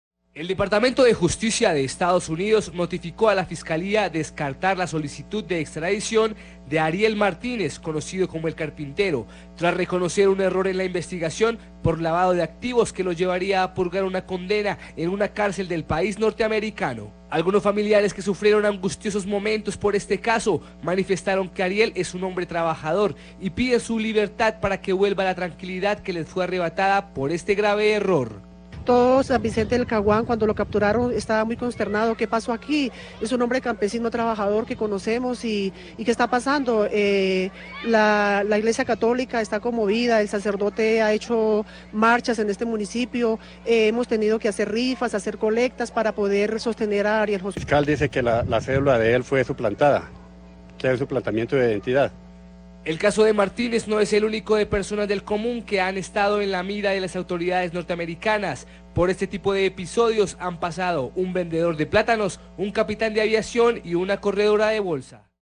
Suspenden extradición del carpintero de Caquetá a Estados Unidos
Las autoridades competentes están llevando a cabo una investigación en contra de Josué Martínez Rodríguez por los delitos de narcotráfico y lavado de activos.

El mismo departamento de justicia de Estados Unidos, gfue quien notificó a la Ficalía colombiana suspender la solicitud de extradición que se había hecho en contra del carpintero de Caquetá, después de conocer que había un supuesto error en la investigación que lo involucra con el delito de lavado de activos y narcotráfico.